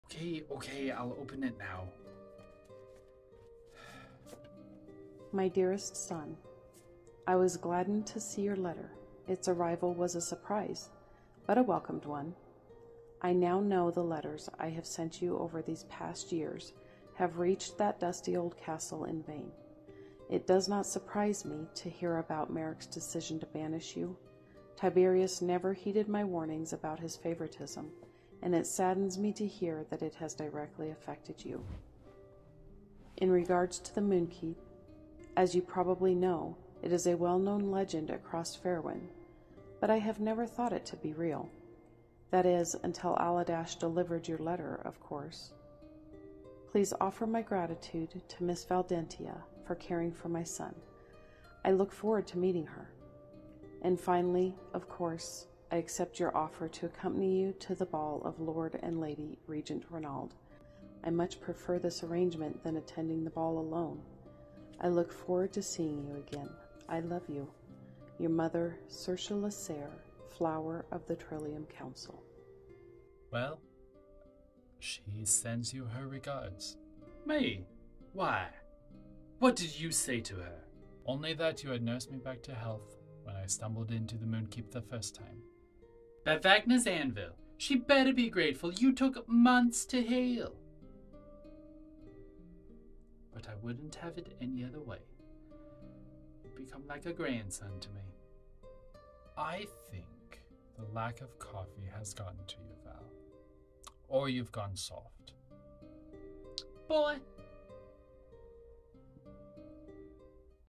Felix gets a letter part 5
Part 5 of the "Felix gets a letter" skit series.

Big, huge, HUMONGOUS shout out to my IRL mother for helping me with this.  She is the voice of Felix's mother, Saoirse La'Saer.